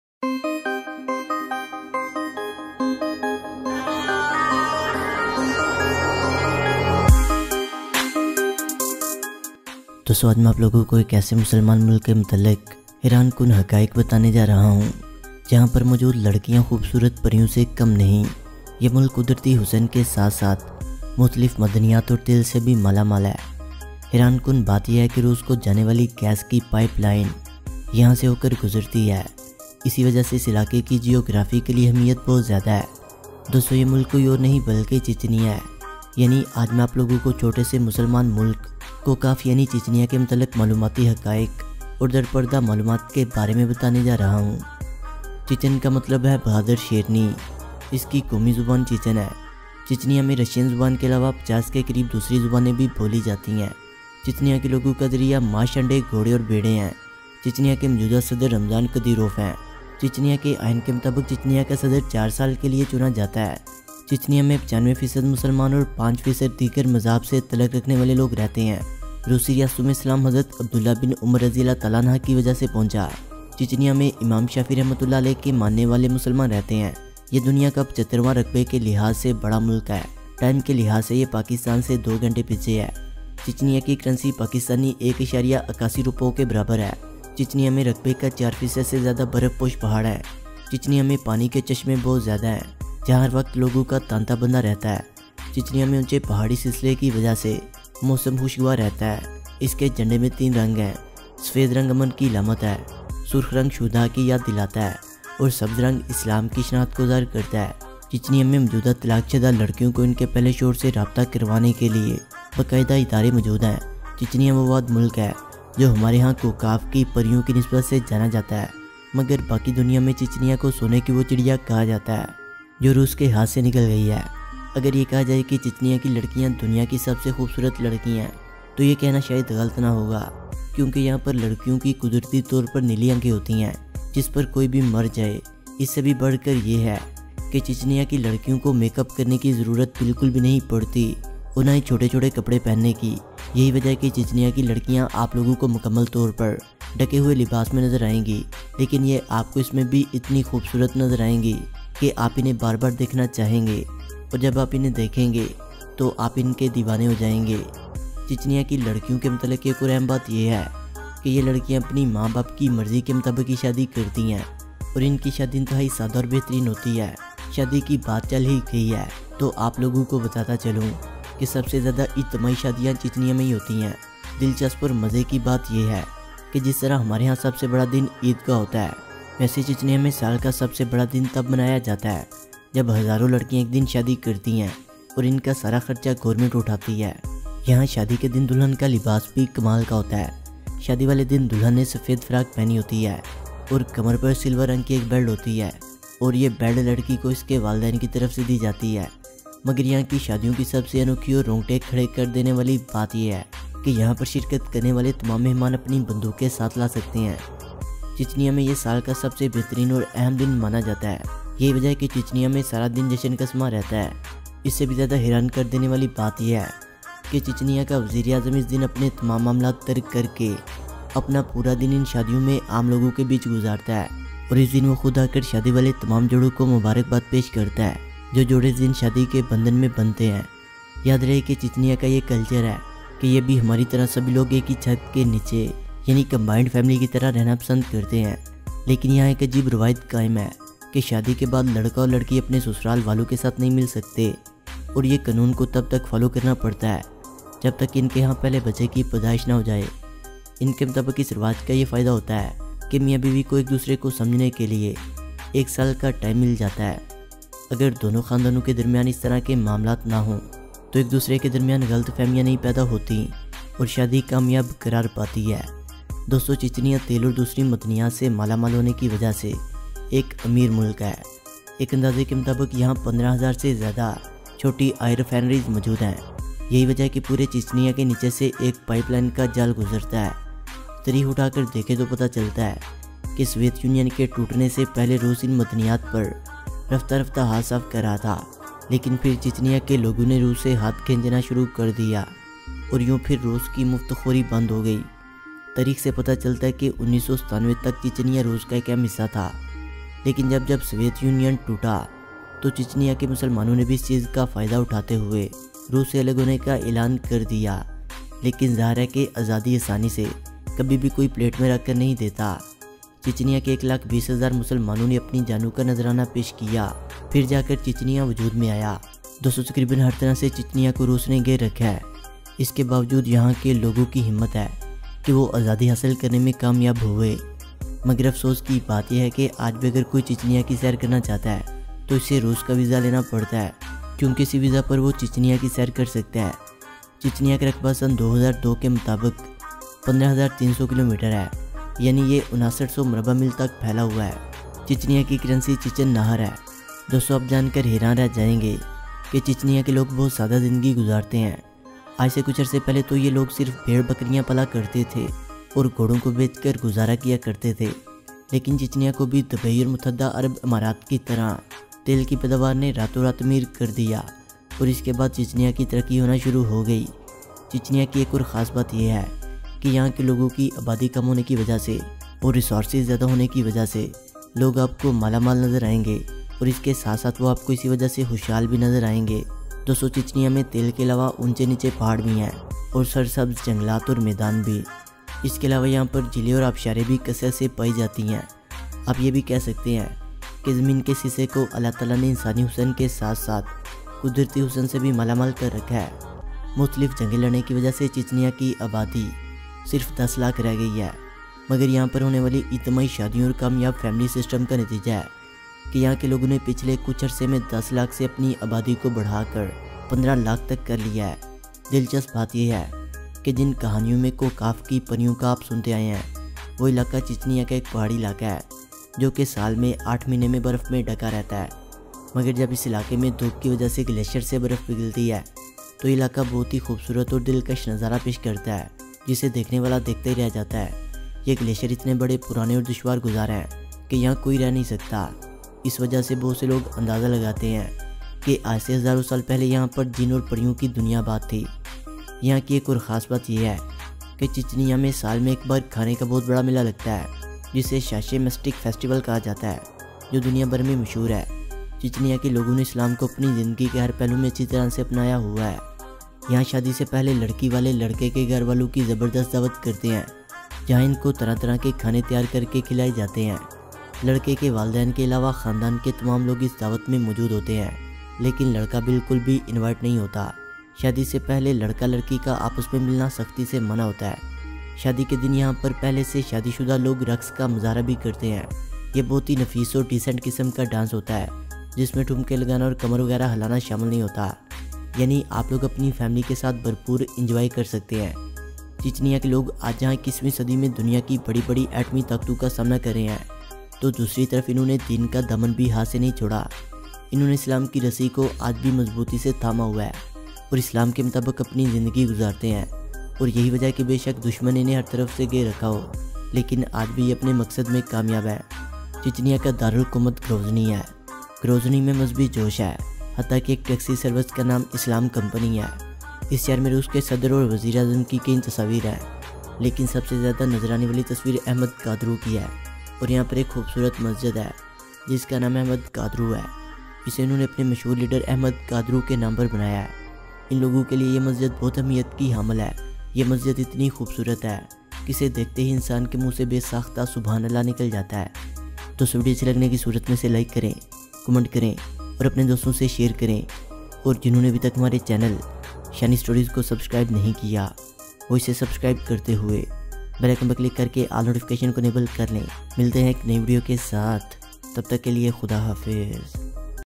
Facts About Chechnya republic | Travel To Chechnya republic | Shani Stories
Chechnya, officially the Chechen Republic, is a constituent republic of Russia situated in the North Caucasus in Eastern Europe, close to the Caspian Sea.